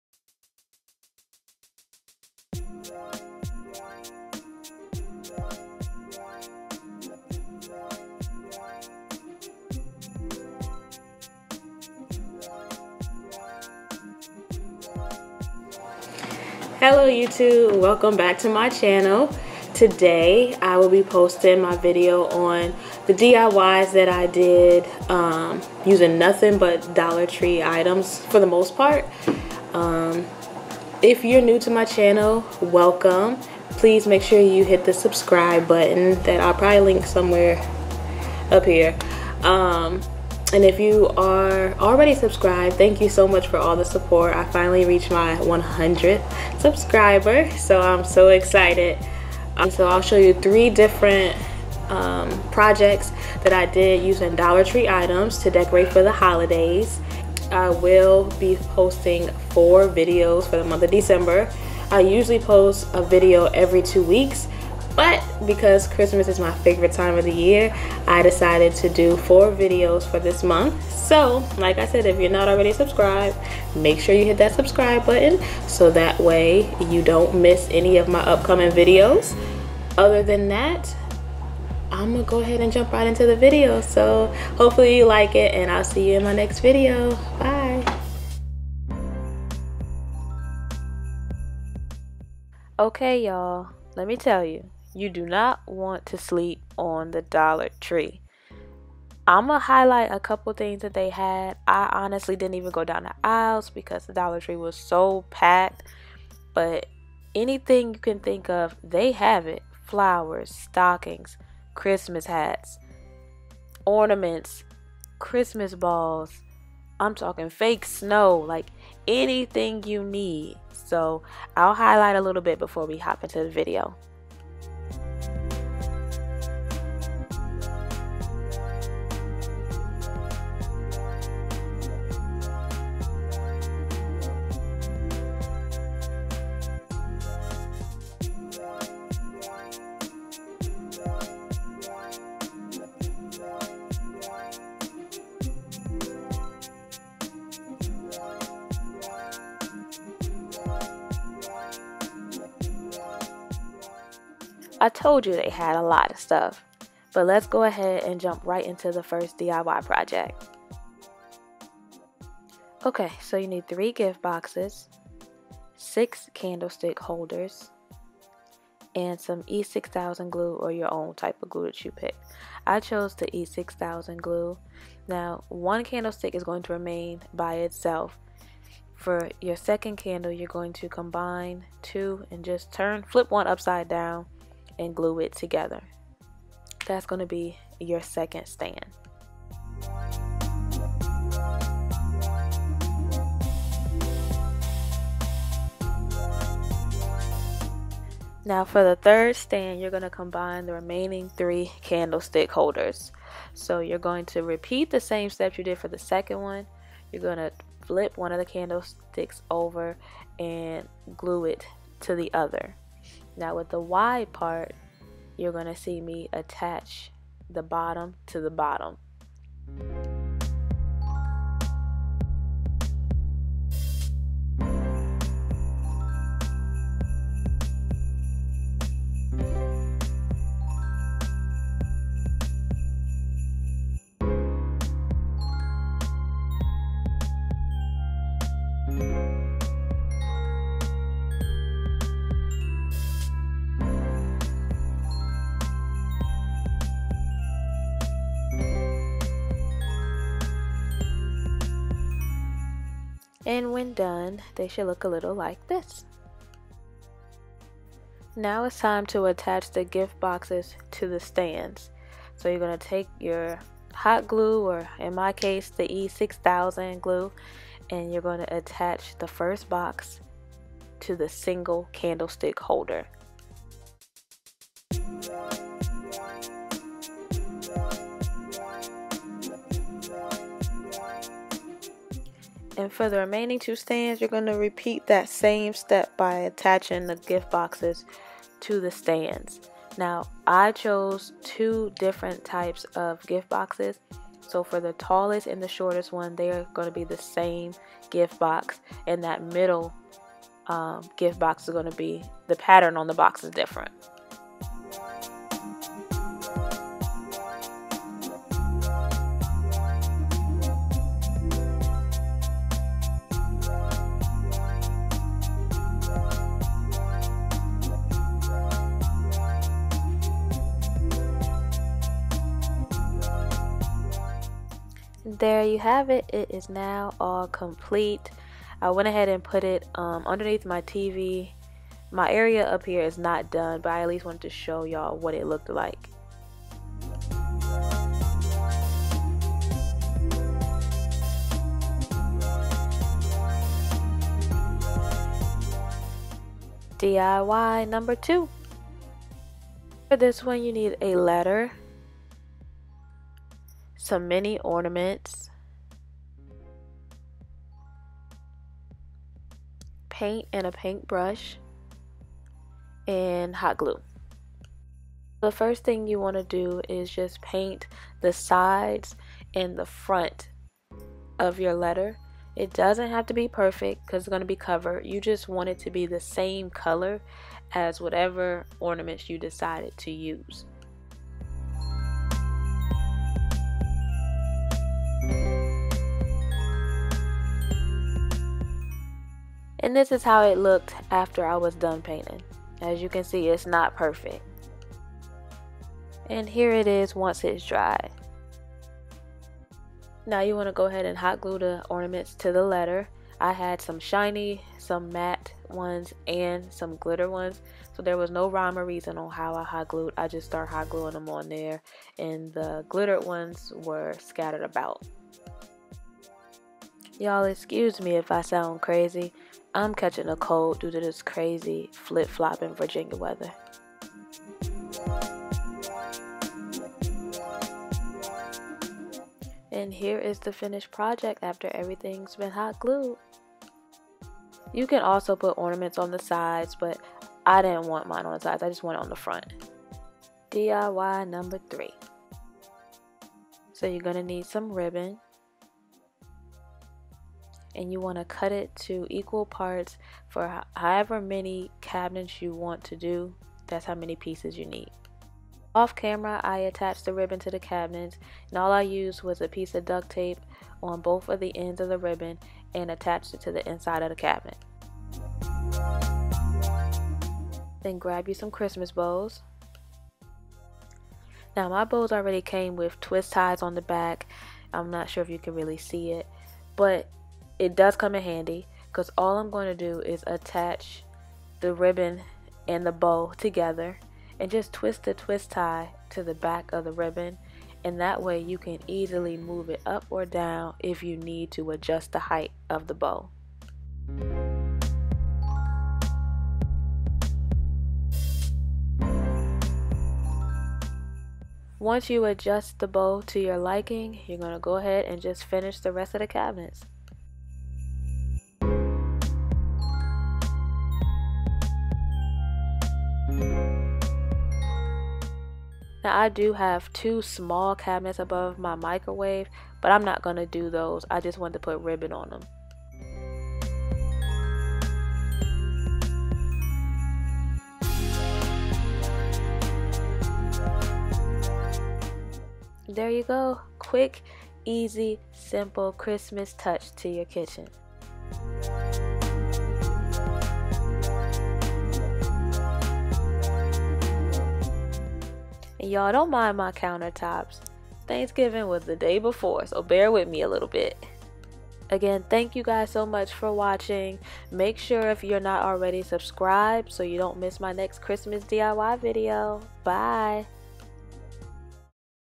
DIY | DOLLAR TREE CHRISTMAS DECOR PART 1
Happy Holidays YouTube Family !!

This video is part 1 of my holiday series for December. I made 3 different DIY Christmas Decor items using Dollar Tree items.

** Present Statues **
EVERYTHING CAME FROM THE DOLLAR TREE !!

** ORNAMENT LETTER **
Paint, paint brush, glue sticks & glue gun  - I already had
Mini ornaments - Dollar Tree
Letter "R" - AC Moore for $3.99

** Present Cabinets **
EVERYTHING FROM THE DOLLAR TREE !!